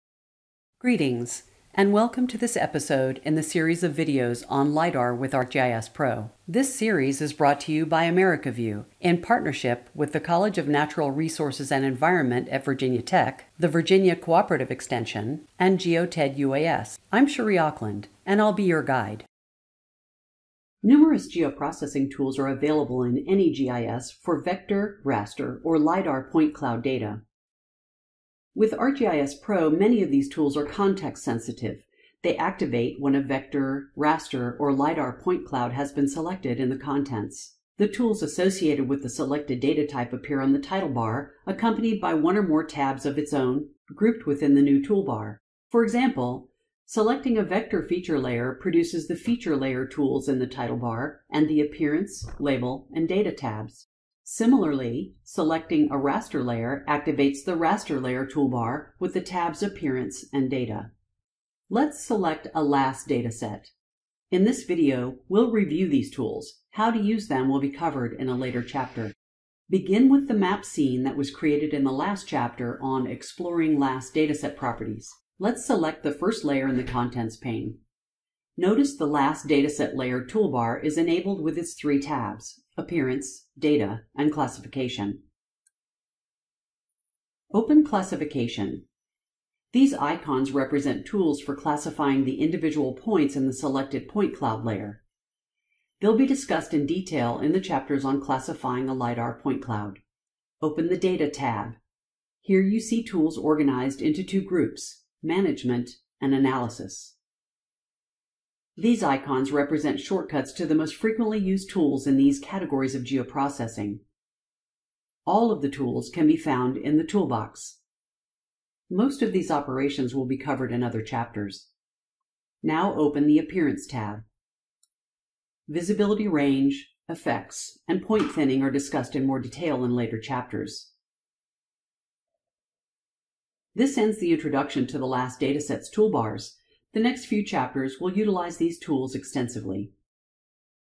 Chapter 12 - Introducing the LAS Dataset Toolbars
This is a companion video for the e-book, "Working With Lidar Using ArcGIS Pro" by Tammy Parece, John McGee and James Campbell.   Video production and narration by Cherie Aukland, GISP.

for $3.99 (also free through Kindle Unlimited). A printed version is also available.   ISBN: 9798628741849